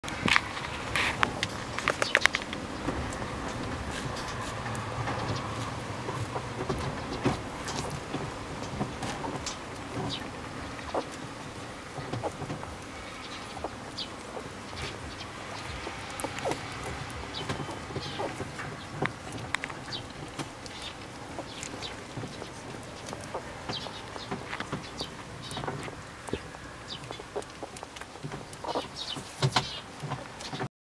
押して押して
オッパイを押して飲む子犬